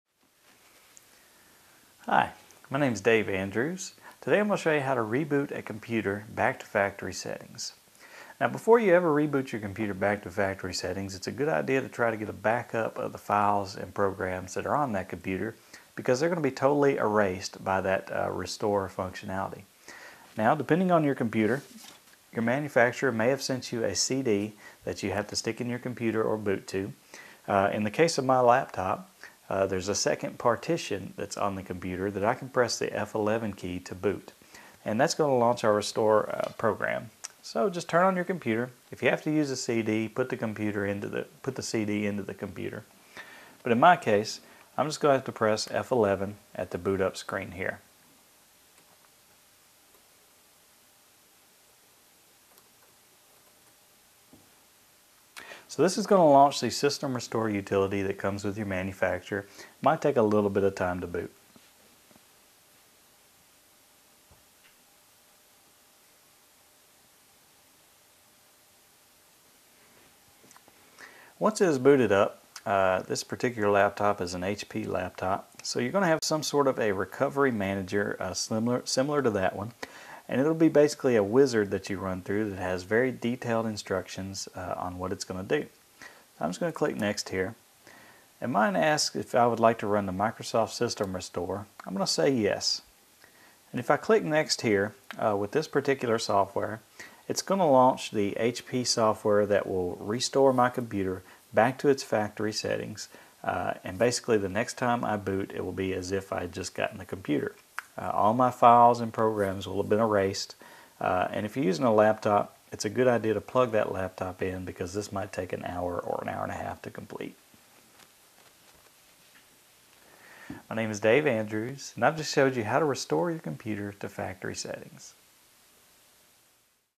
Computer Information : How to Reboot a Computer Back to Factory Settings
Rebooting a computer back to factory settings can be done by inserting the original CD that came with the computer or pressing F11 to open the boot-up screen. Back up all files before rebooting a computer to factory settings with information from an experienced software developer in this free video on computers.